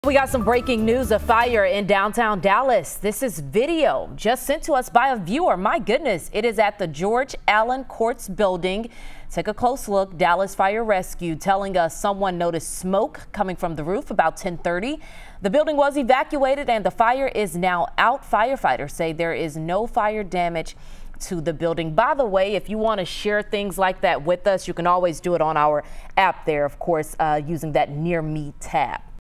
No injuries reported after boiler malfunction caused fire at George Allen Courts Building
Dallas Fire-Rescue crews responded to fire Wednesday morning at the George Allen Courts Building in downtown Dallas.